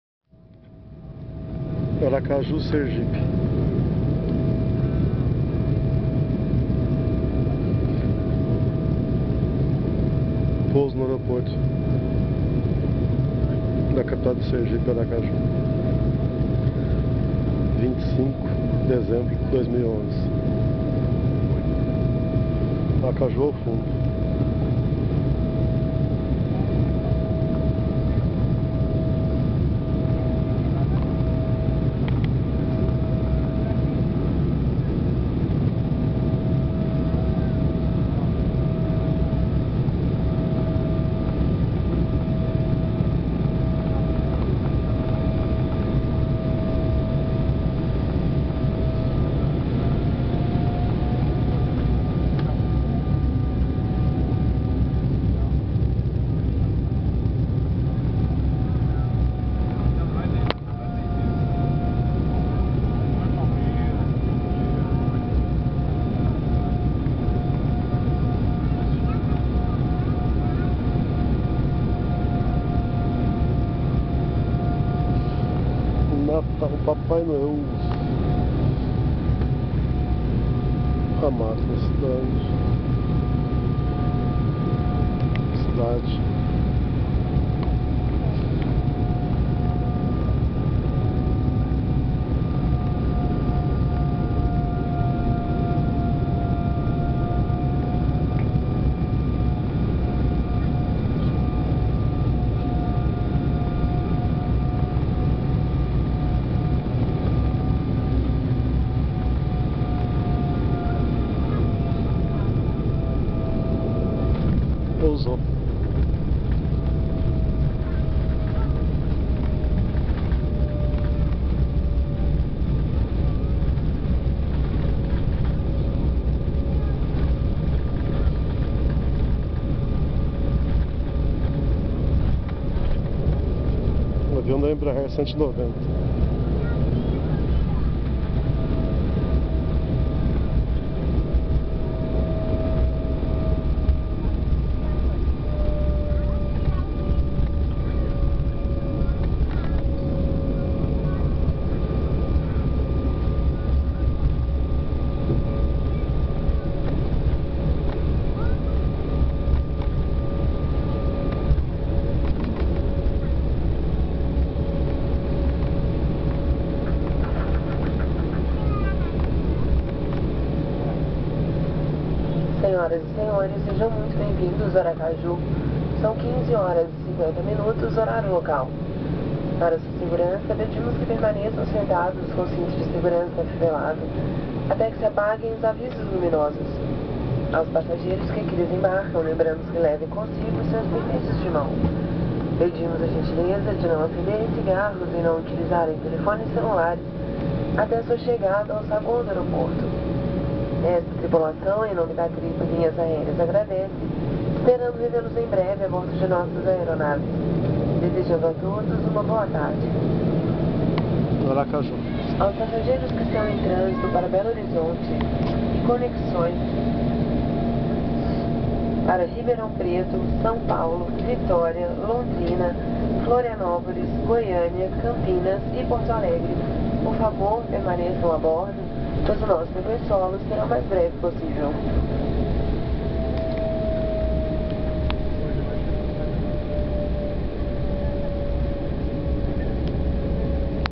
POUSO DO AVIÃO DA TRIP JATO EMBRAER 190, ARACAJU SERGIPE.
POUSO NO AEROPORTO DE ARACAJU SERGIPE, NATAL DE 2011, VOO PANORÂMICO SOBRE CIDADE DE ARACAJU, EM 25/11/2011.